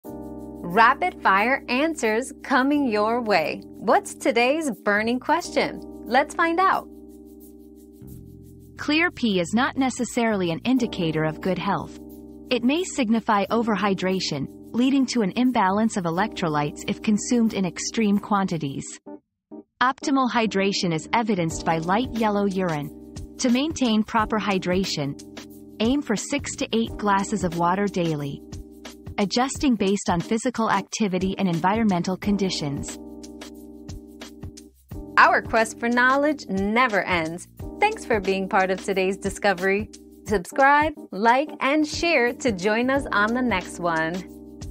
Is clear pee good?
Clear Pee: Good or Bad? 👉 Clear Pee Myth 👉 Discover the truth about clear pee and what it really means for your health. Learn how to stay properly hydrated for optimal wellness.

Laura S. Harris (2024, April 8.) Is clear pee good?
    🌐 AskAbout.video